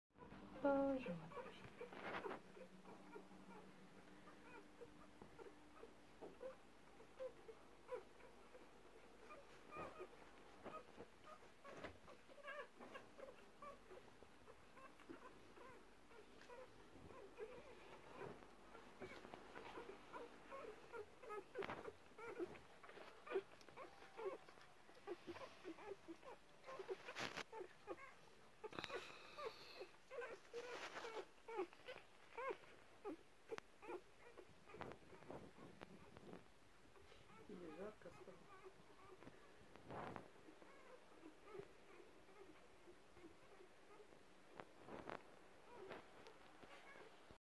Щенки Бернского Зенненхунда. Первые сутки
В общем роды прошли на отлично! Собака родила сама. Первый щеночек родился 16 января в 5.45 утра и 8-й в 18.35)